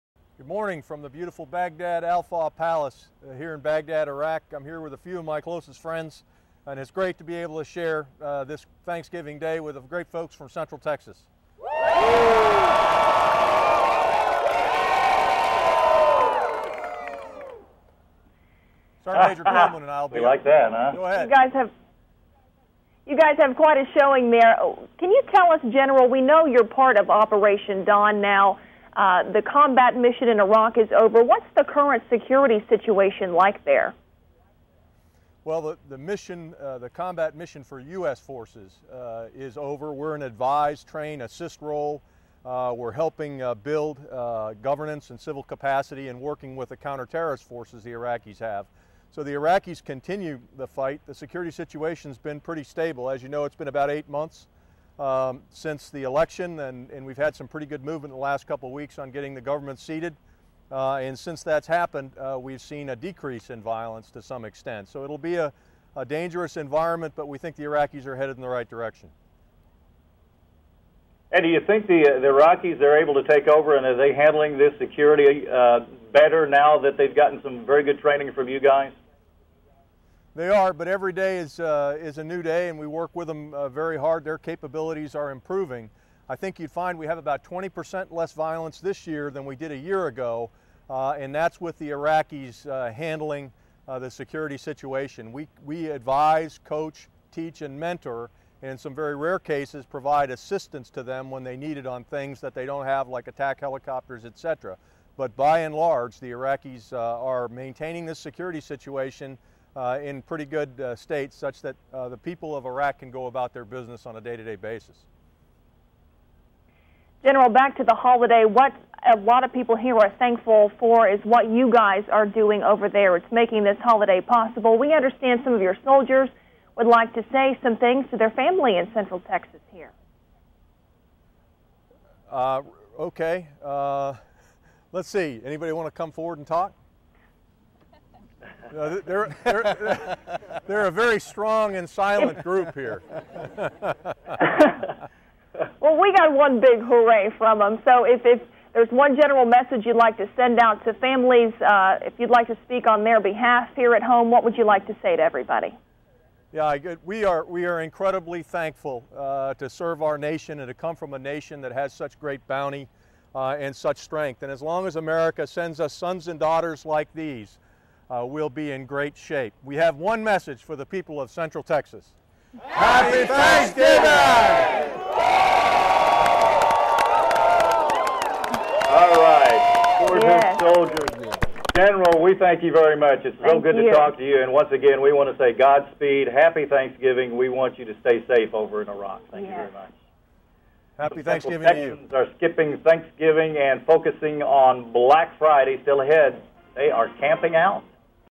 Thanks giving message.wmv
Lt. Gen. Bob Cone, III Corps Commander and Command Sgt. Maj. Arthur Coleman, III Corps Command Sgt. Maj., talk to a KCEN-TV reporter in Waco, Texas, from Al Faw palace in Baghdad, Iraq, and send a Thanksgiving message to the Central Texas community.